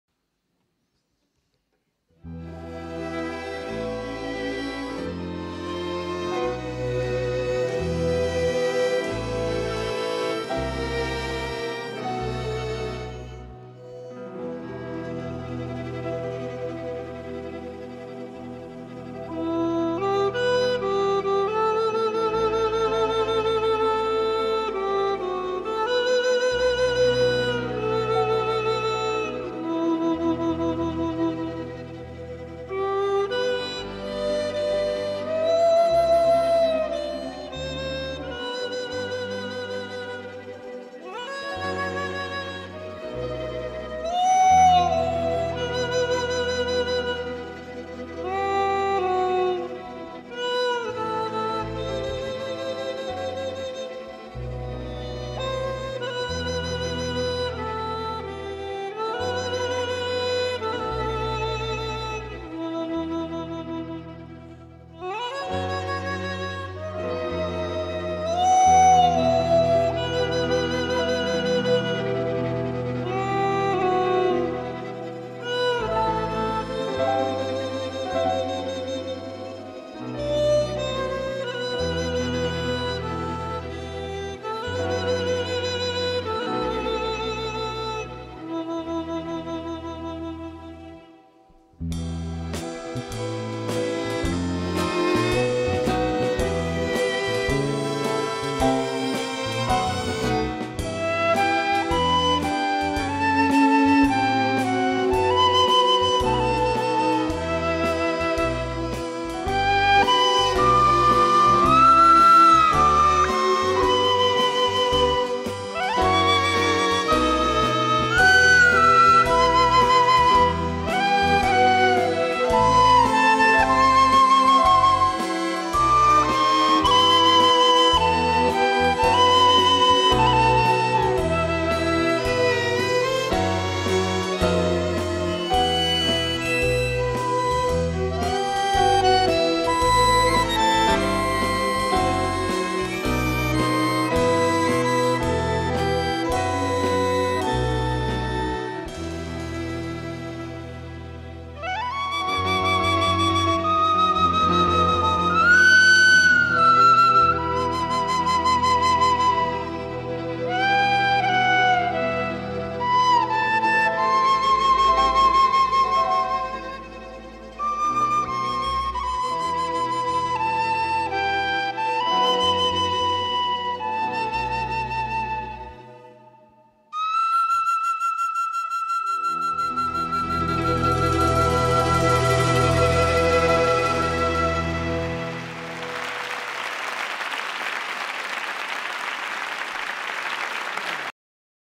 Bora Dugic Rusija u pesmi i igri
Muzička produkcija  MAESTRO INTERNATIONAL
80 godina postojanja Ruskog doma u Srbiji
Sava centar,maj 2013